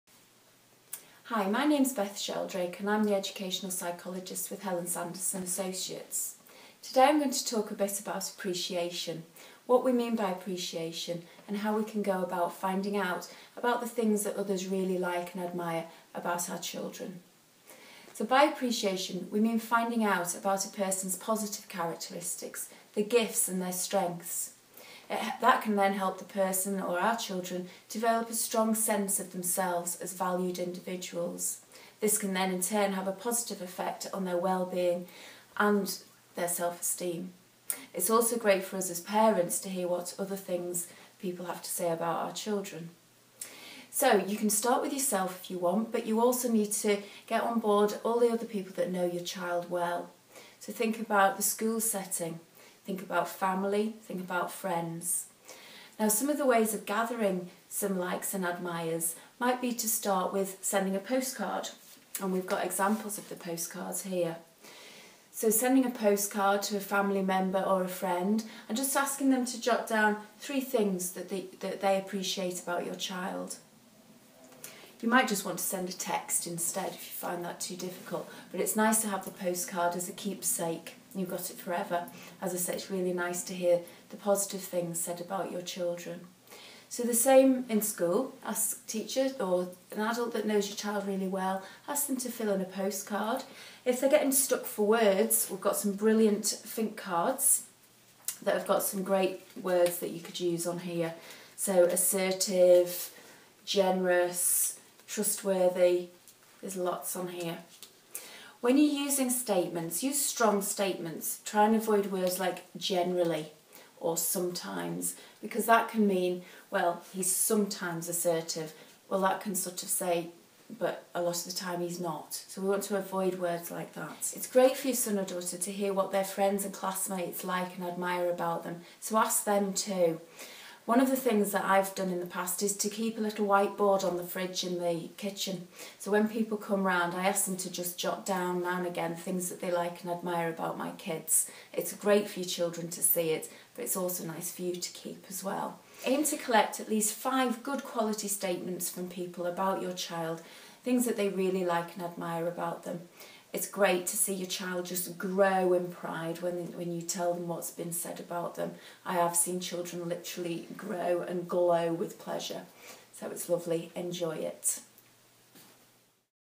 Appreciation
Beth Sheldrake, Educational Psychologist with Helen Sanderson Associates, shares ways to develop appreciations for your child’s one-page profile.